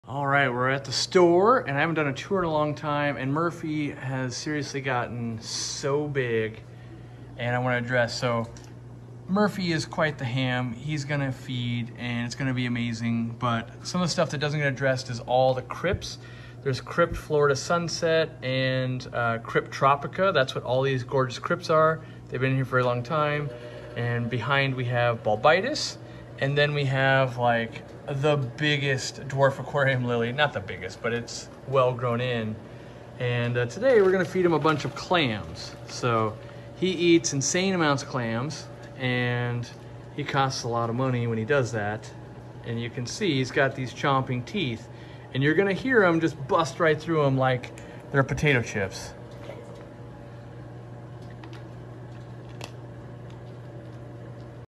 Largest Freshwater Puffer Species
This is Murphy our store mascot. He loves to ham it up on camera and eat clams.